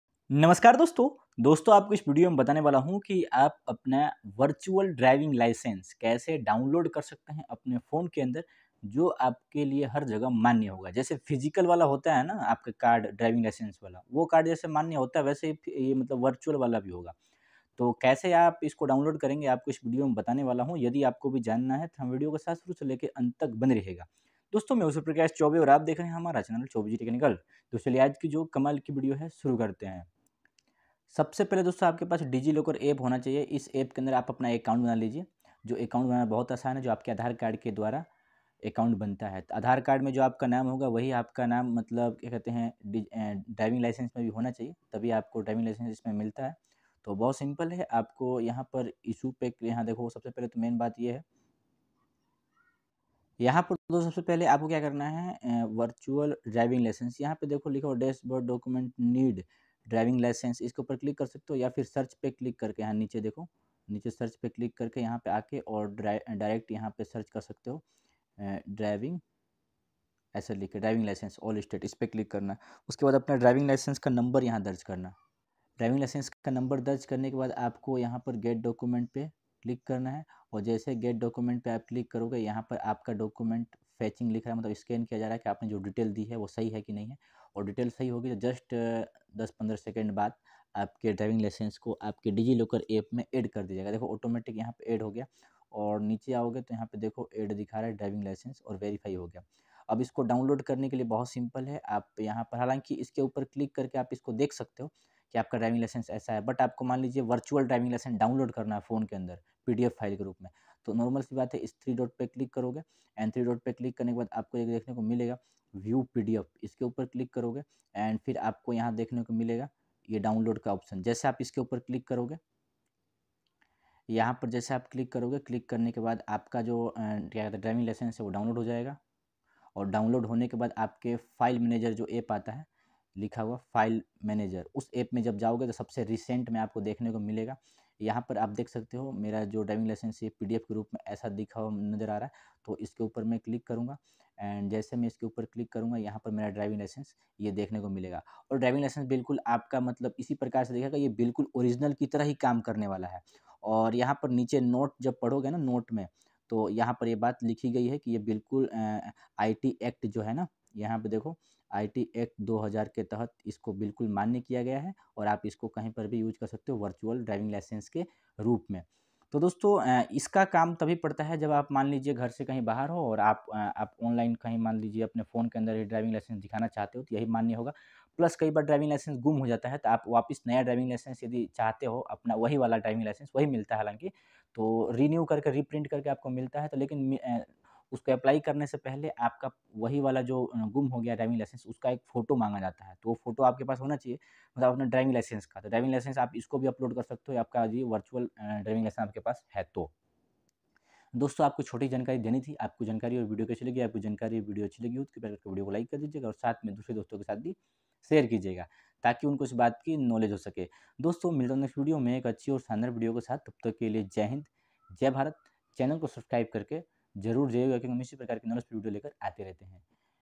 Virtual driving license kaise download kare online | digital driving license download online
Hello friend today I'm describe that in this video how to download virtual driving license online 2024 in hindi.

Hero choubey